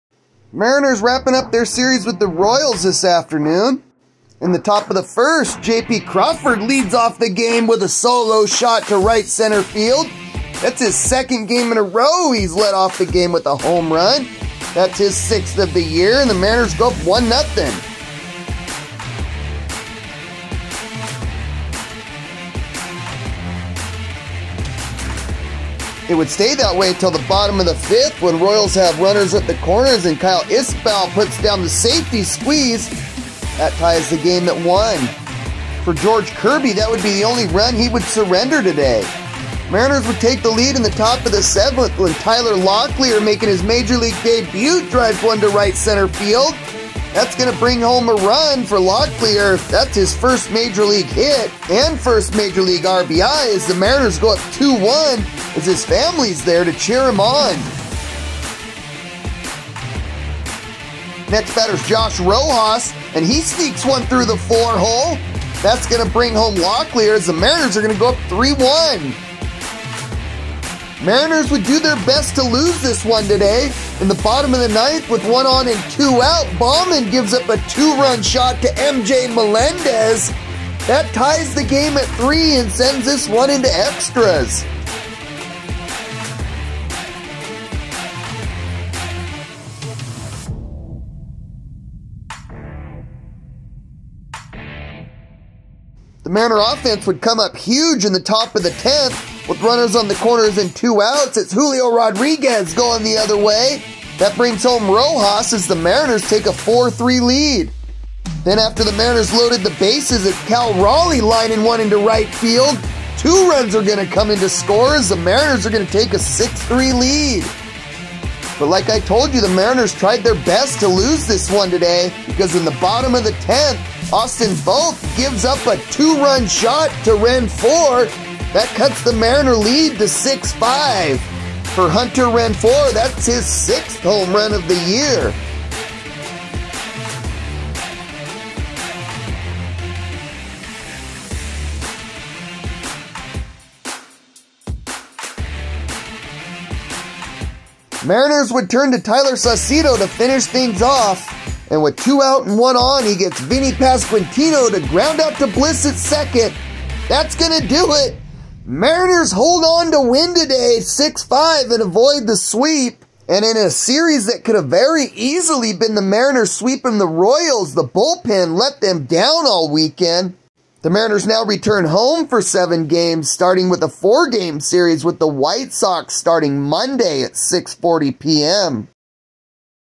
Seattle Mariners at KC Royals Highlights
6.9.2024
Game #67
Seattle Mariners at Kansas City Royals
2024 Seattle Mariners Highlights
MLB Highlights
2024 Major League Baseball

Final -10
Mariners -6
Royals -5

Rodríguez hits tiebreaking single in 3-run 10th as Mariners outlast Royals 6-5 to avoid 1st sweep

By AP

KANSAS CITY, Mo. -- — Julio Rodríguez hit a tiebreaking single in the 10th inning, Cal Raleigh added a two-run single and the Seattle Mariners held off the Kansas City Royals 6-5 on Sunday to avoid getting swept for the first time this season.

J.P. Crawford hit his second consecutive leadoff homer and George Kirby pitched seven splendid innings for the AL West leaders, who squandered leads while losing the first two games of the weekend series.

“It’s a tough place to win a ballgame,” Mariners manager Scott Servais said. “The Royals are pesky as hell. We needed all the extra cushion that Cal provided with the big hit there.”

Seattle blew a late lead again Sunday but recovered to halt Kansas City’s three-game winning streak.

MJ Melendez hit a two-run homer off Mariners reliever Mike Baumann (3-0) with two outs in the ninth to tie it 3-all.

“MJ was ready to hit,” Royals manager Matt Quatraro said. “Really good approach. Looked like a fastball, pretty much middle, and he was ready to go.”

With runners at the corners and two outs in the 10th, Rodríguez singled to right field off James McArthur (2-3) to break the tie. Mitch Garver walked and Raleigh followed with a two-run single to make it 6-3.

“It was huge to squeak one out today,” Raleigh said. “We needed that one.”

Hunter Renfroe led off the bottom half with a two-run homer against Austin Voth, and Kyle Isbel singled. Taylor Saucedo entered with two outs and got Vinnie Pasquantino on a game-ending grounder for his third save.

Making his major league debut, Tyler Locklear broke a 1-all tie with a seventh-inning double and scored on pinch-hitter Josh Rojas’ single to give Seattle a 3-1 lead.

“I thought (the double) was going to hang up a little bit,” Locklear said. “Just glad that ball got down and we got a run out of it.”

Kirby permitted just two hits over the first four scoreless innings. Freddy Fermin and Renfroe hit consecutive singles in the fifth before Isbel’s squeeze bunt tied the game.

“They’re aggressive,” Kirby said. “You’ve got to really try to be precise with the slider. They were swinging at everything, so just I focused on location.”

Kirby completed seven innings, allowing a run on five hits with four strikeouts.

“He established his fastball inside,” Servais said. “We needed all seven innings from him today. He stood up and did exactly what we needed.”

Crawford led off with a homer for the second straight game, his 12th career leadoff shot and fifth since 2023 — most in the majors.

It was the first home run given up by Royals starter Cole Ragans since May 11.

“Just got to move on,” Ragans said. “Lot of game left. The guys we have can put up crooked numbers.”

Ragans retired 12 of the next 13 batters — eight by strikeout — and finished six innings. He allowed just one run on three hits and a walk, fanning nine.